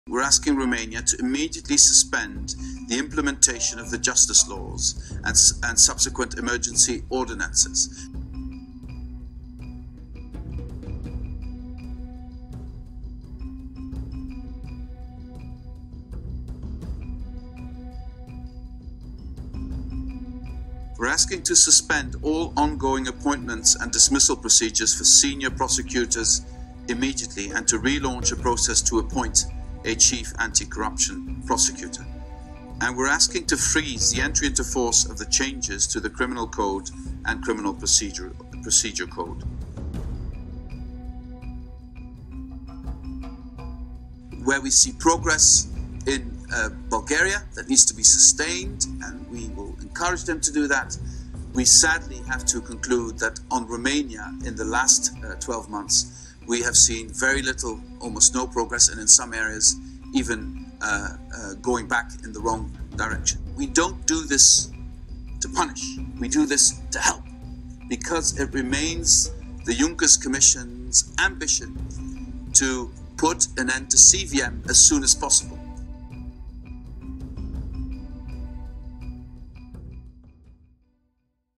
Cel mai dur Raport MCV pentru România
A fost publicat raportul MCV (Mecanismul de Verificare şi Cooperare în Justiţie) pentru România care este extrem de critic la adresa Guvernului şi Parlamentului. Principalul capăt de acuzare: atacurile la justiţie.